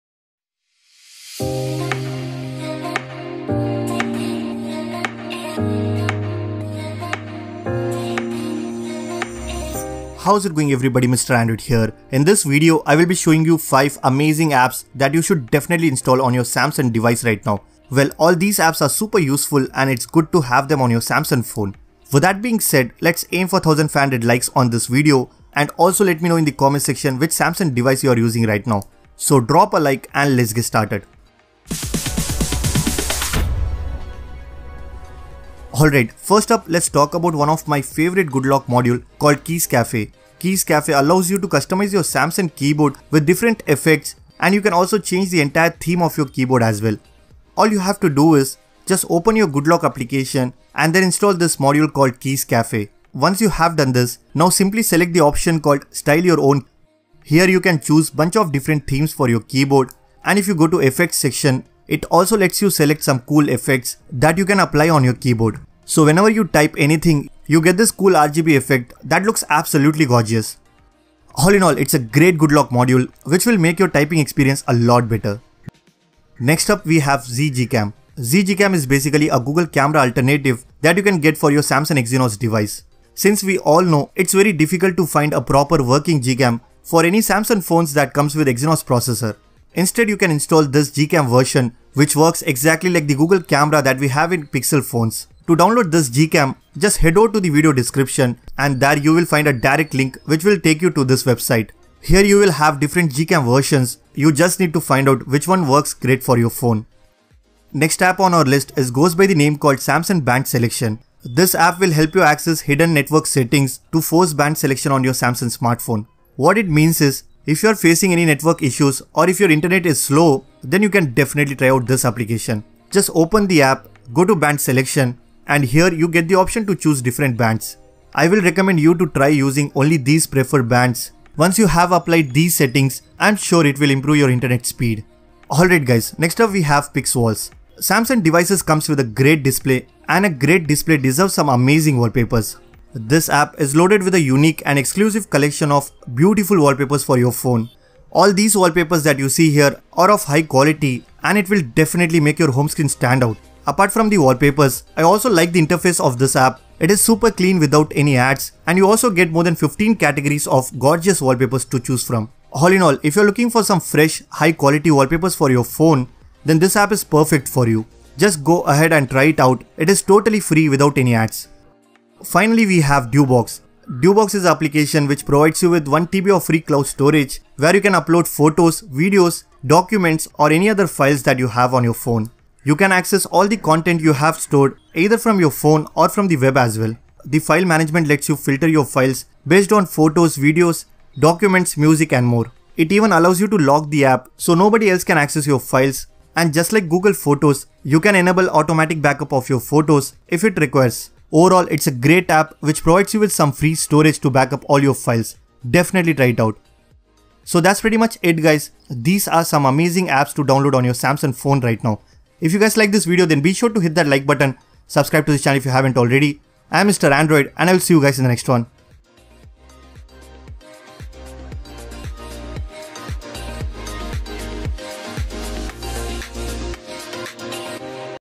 5 Best Exclusive Apps Made For All Samsung Galaxy Devices - 2021
5 Best Exclusive Apps Made For All Samsung Galaxy Devices - 2021

In this video, I will show you 5 Best Exclusive Apps Made For Samsung Galaxy Devices. All these apps are very useful and must have for your samsung smartphone in 2021.

Please do join our Telegram Group for some exclusive wallpapers and tech news:

Apps link (As per video order)